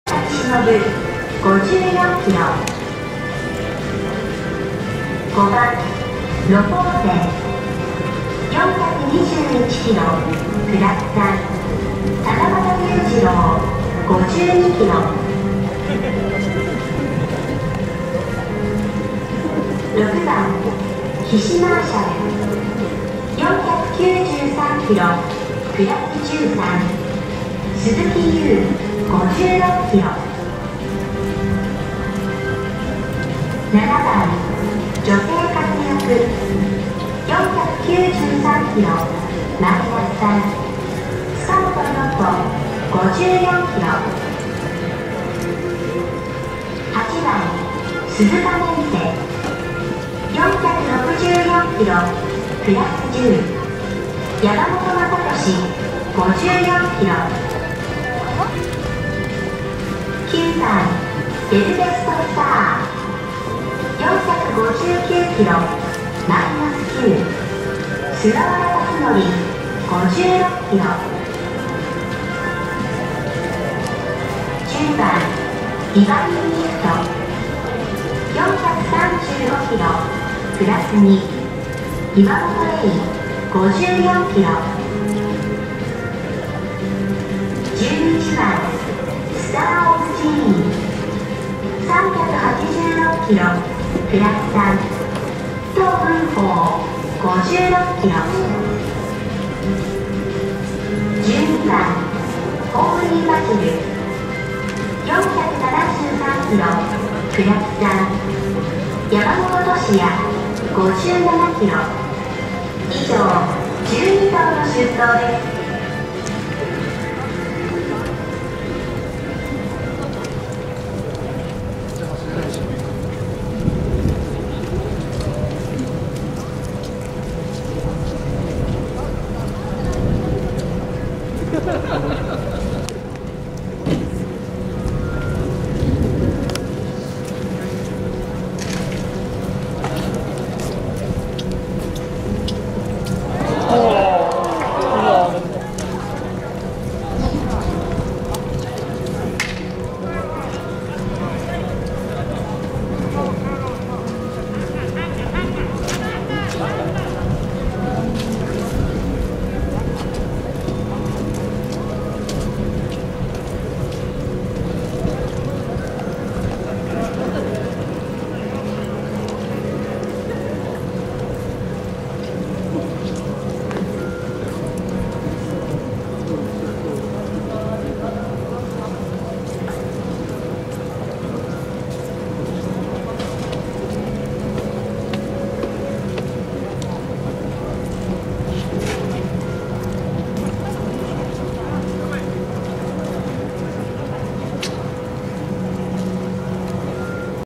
2021 南部杯デーの5レースで起こった衝撃的な出来ごと 現地映像
2021
南部杯デーの5レースで起こった衝撃的な出来ごと現地映像です。
馬名がジョセイカツヤク 騎手が塚本涼人
この事がキッカケで塚本涼人は以降のレースは負傷のため騎乗変更となりました。